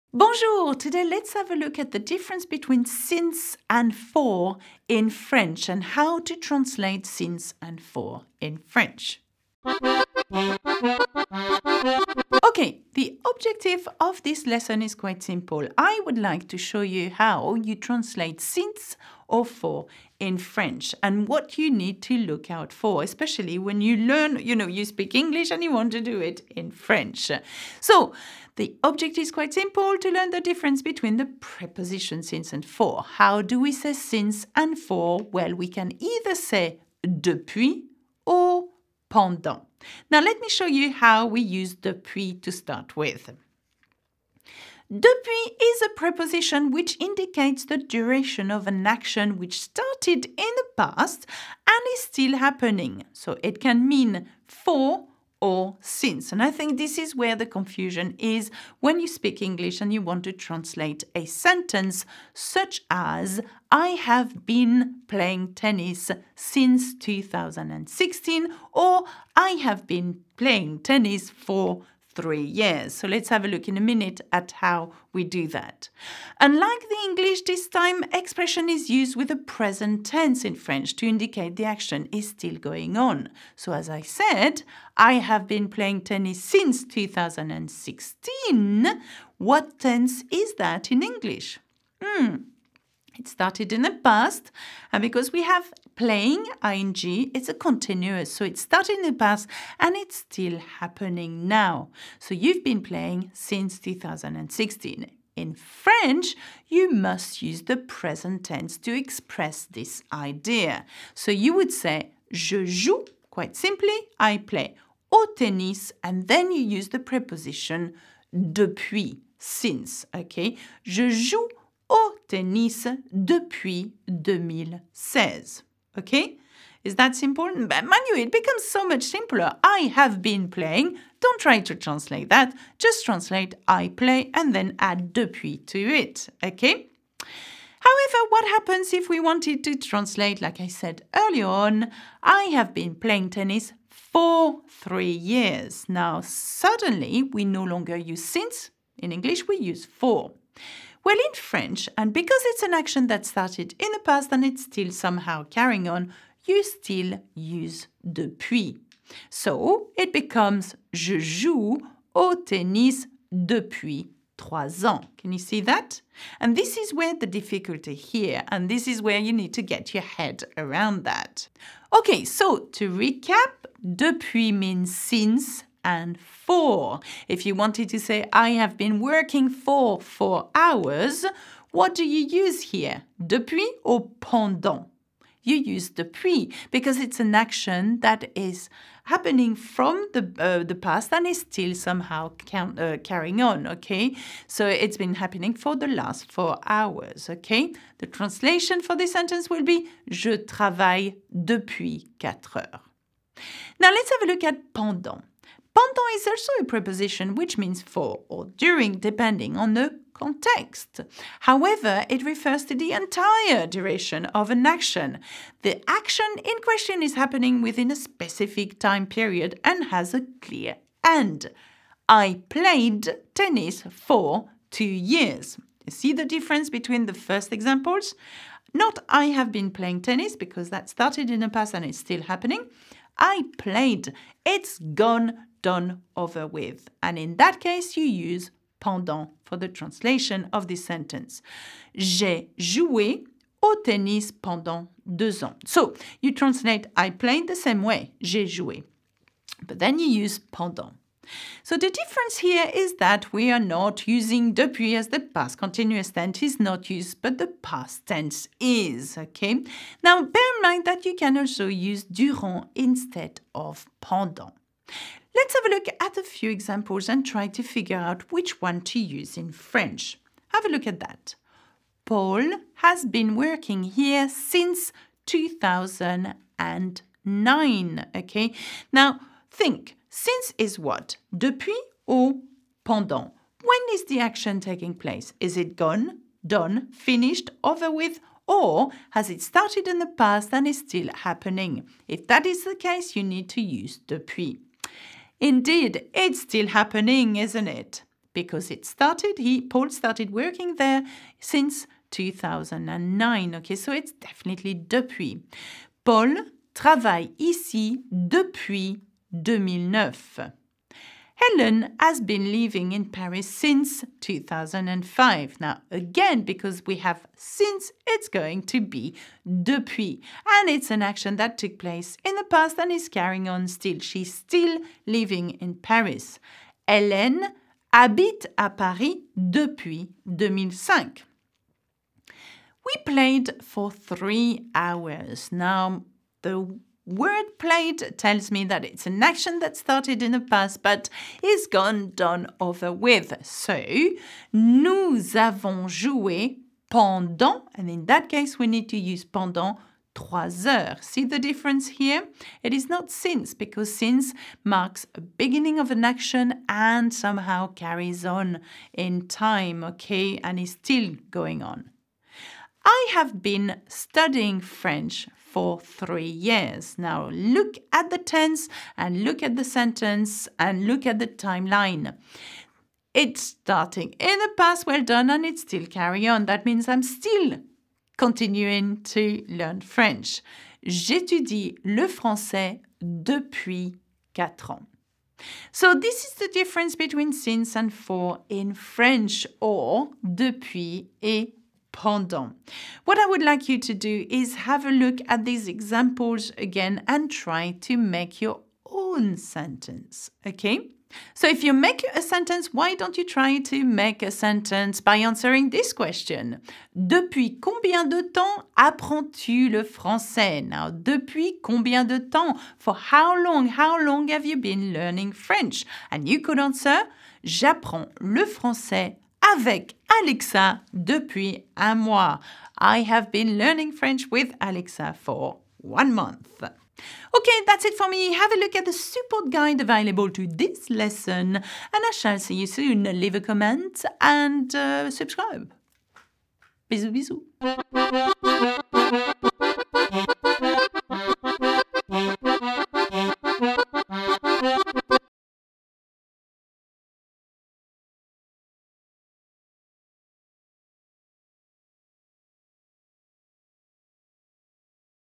DEPUIS or PENDANT? How to say SINCE and FOR in French.
Alexa teaches you how to say SINCE and FOR in French.

Bisou Bisou 💋

0:00 - 0:39 Intro
0:40 - 3:22 Depuis
3:23 - 4:24 Pendant
4:25 - 7:06 Depuis vs pendant in French
7:07 - 7:35 Outro